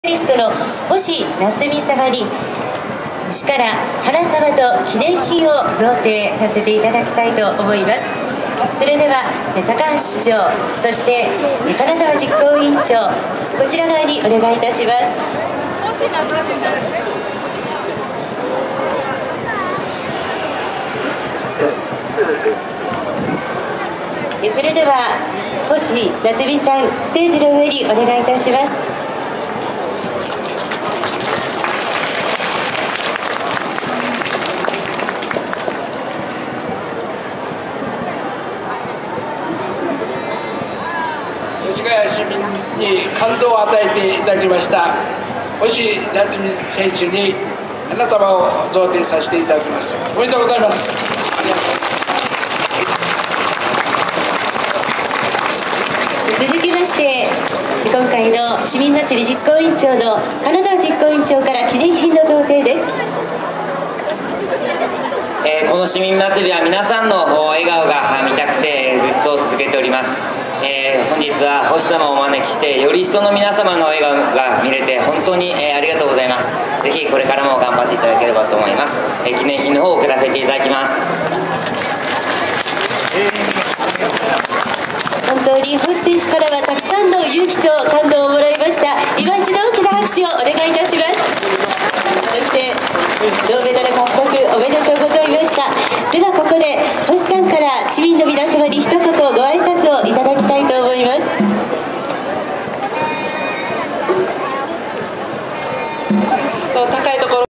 星奈津美選手（ロンドン銅メダリスト ）へ花束と記念品贈呈
2012年10月21日（日） 第38回越谷市民まつり

ロンドンオリンピック 女子２００メートルバタフライ銅メダリストの星奈津美選手に、越谷市長から花束が、市民まつり実行委員長・金沢重和氏からは記念品が贈呈された。